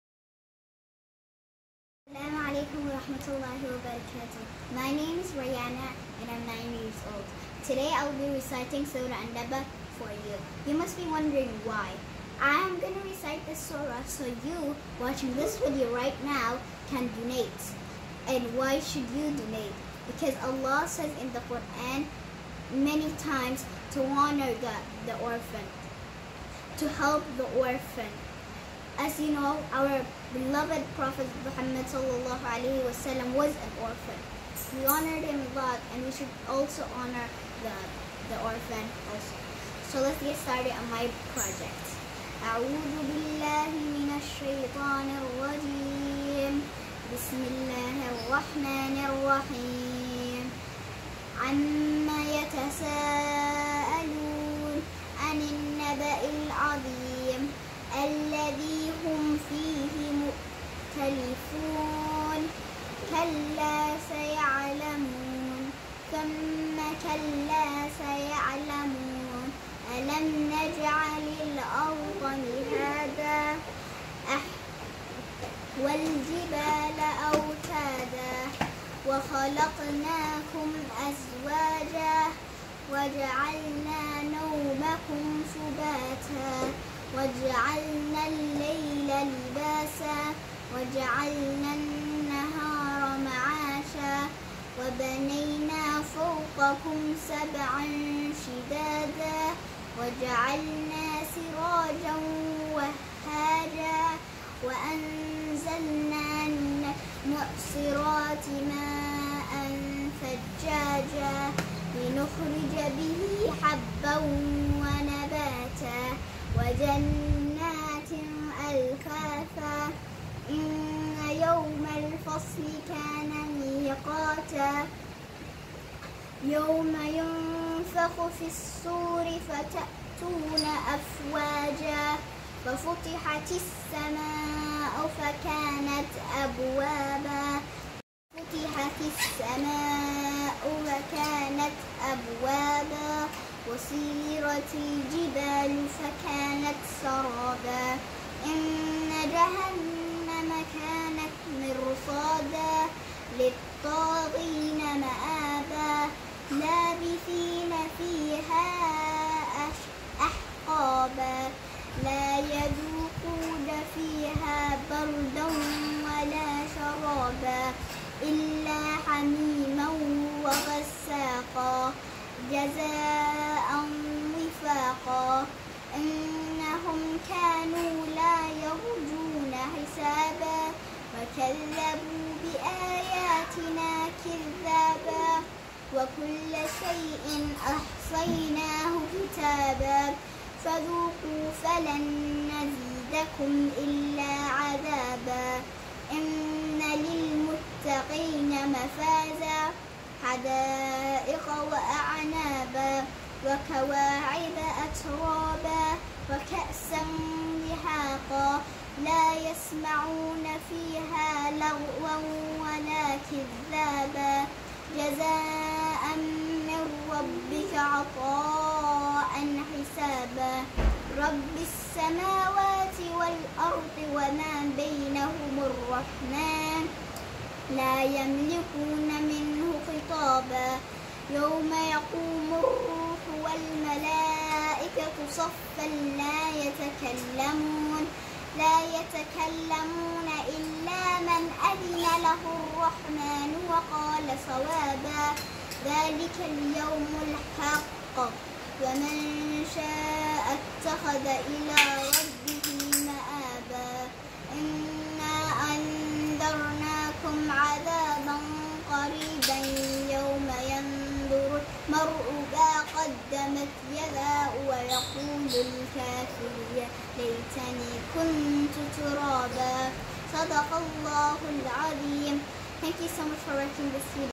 Rayana's pledge for Orphans
Please support Rayana's pledge for orphans by donating generously.

Emaan Power students and Muslim children from across the world have not forgotten their brothers and sisters.

They are making pledges that they would love for you to sponsor, so they can raise money for the orphan children.

They took pledges to read quran. fast and memorise Quran, this ramadan to raise money for th Orphans (raised for Human Appeal International)

MashaAllah they have been working hard to fulfill their pledges and now its your turn to show them support.

Please show these young hearts that the ummah is behind them and their efforts are not in vain

Please support the children in their efforts and earn great amount of Sadaqa Jariya.